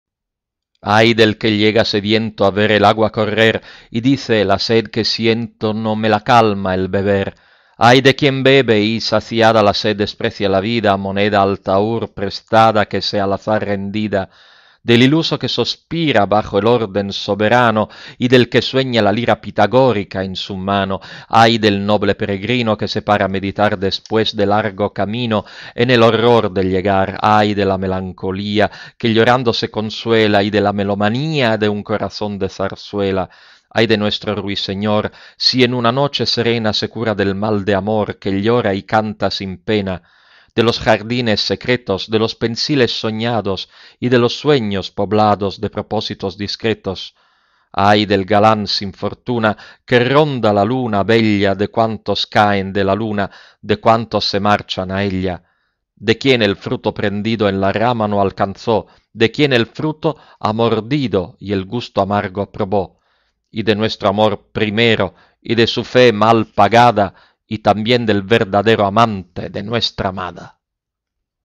Antonio Machado, Ay del que llega sediento, Audiolibro, Valerio Di Stefano
de "Soledades, , Galerías. Otros Poemas" (1907)